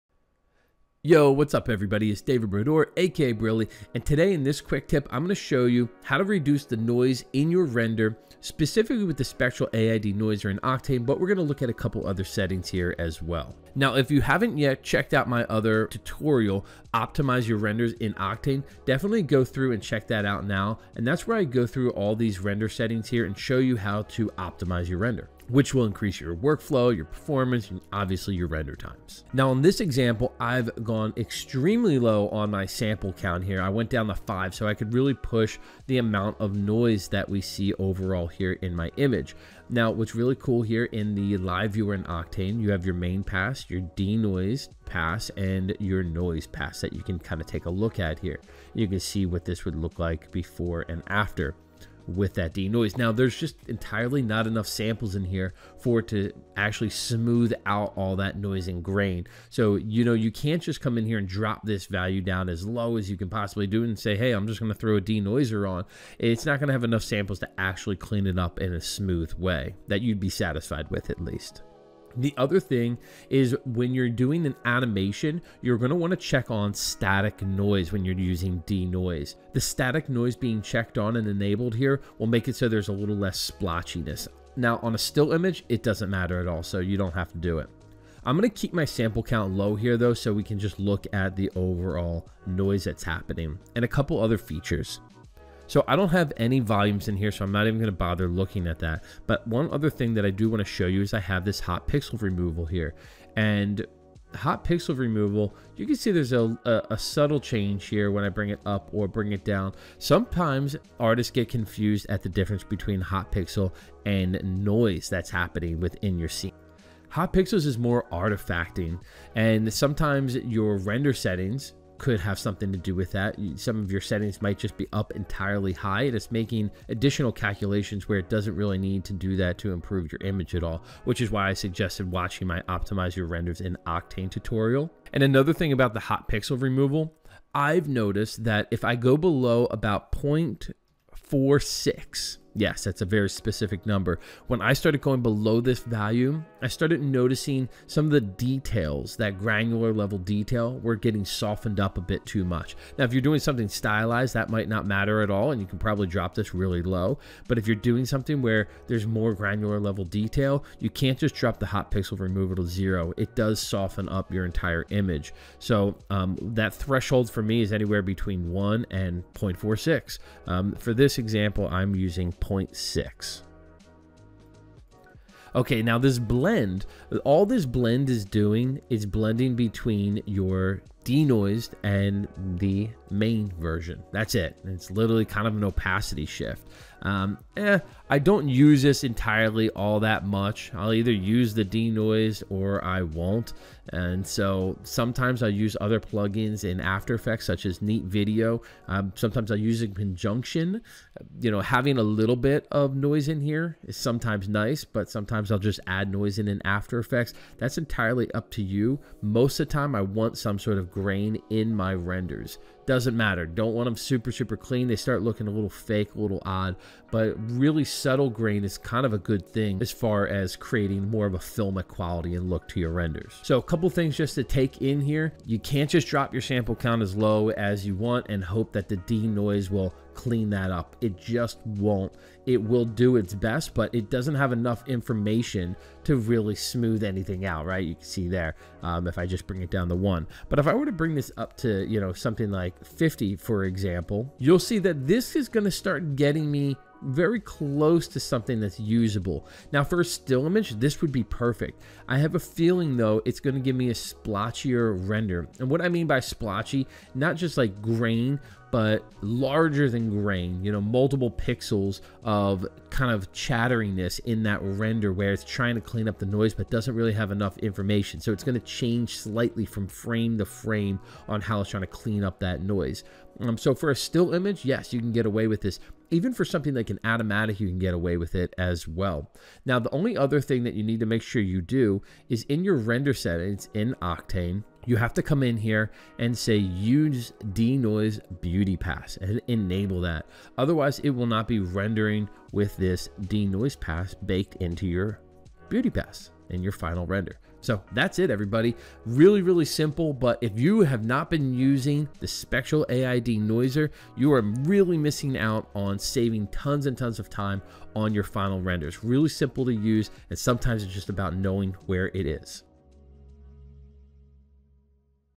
Remove Noise From Your 3D Render
Clean up your renders fast with Spectral AI Denoiser built into Octane Render.

In this Quick Tip tutorial, David Brodeur AKA Brilly shows you how to set up denoising and a couple other noise removal techniques using Octane Render.